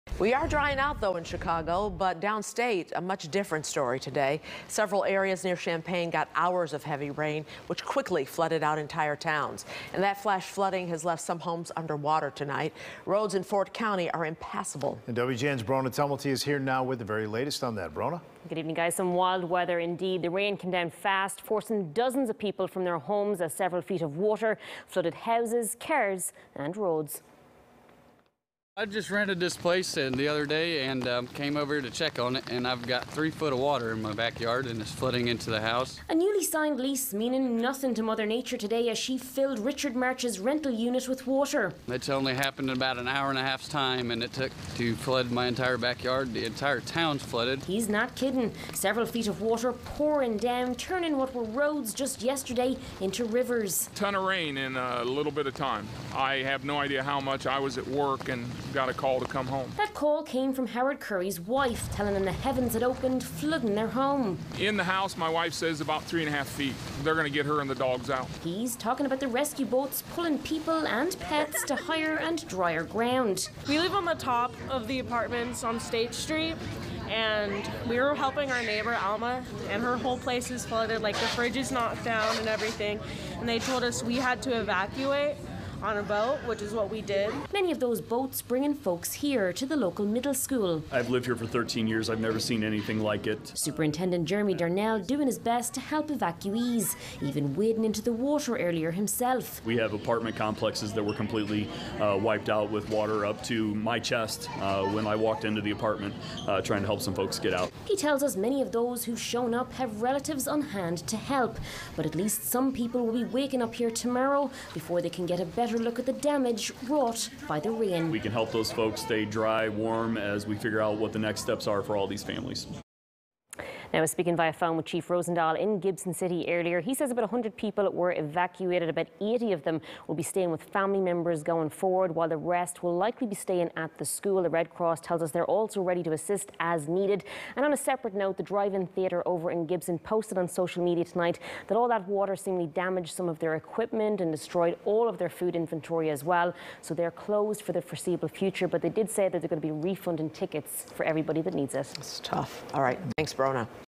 More than 9 inches of rain drenches downstate Gibson City
Water rescues and evacuations are underway after heavy rainfall caused flash flooding across Gibson City.